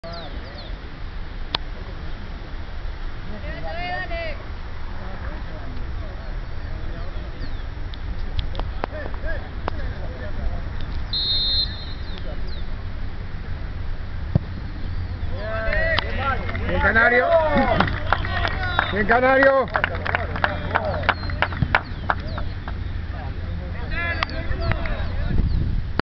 Gol de Valentín Raymundo a Miramar Misiones
Torneo Sub 16
Campeonato Clausura 2011
Liverpool 3 Miramar Misiones 0
Gol de Valentín Raymundo